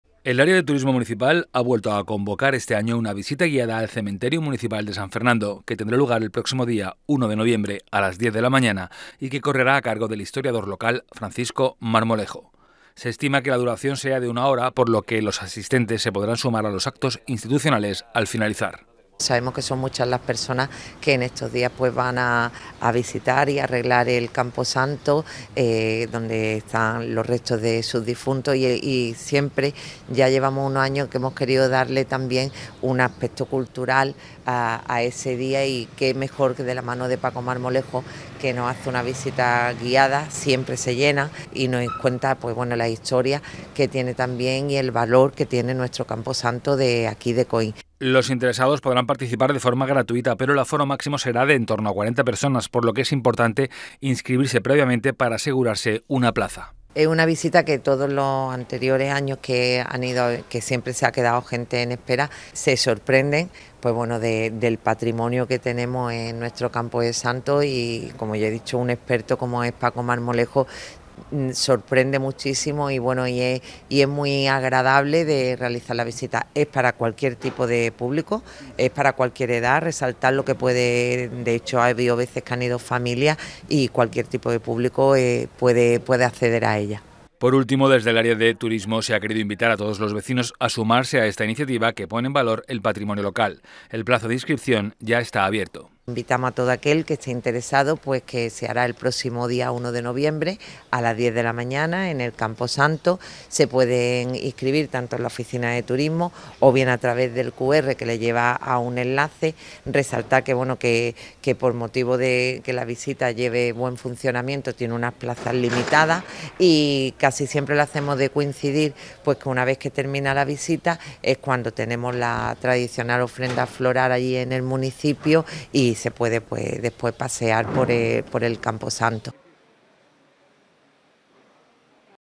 ANUNCIO RUTA GUIADA CEMENTERIO
El área de Turismo municipal ha vuelto a convocar este año una visita guiada al cementerio municipal de San Fernando, que tendrá lugar el próximo 1 de noviembre a las diez de la mañana y que correrá a cargo del historiador local Francisco Marmolejo. Se estima que la duración sea de una hora, por lo que los asistentes se podrán sumar a los actos institucionales al finalizar.
Los interesados podrán participar de forma gratuita, pero el aforo máximo será de en torno a 40 personas, por lo que es importante inscribirse lo antes posible para asegurarse una plaza.
Por último, desde el área de Turismo se ha querido invitar a todos los vecinos a sumarse a esta iniciativa que pone en valor el patrimonio local. El plazo de inscripción ya está abierto.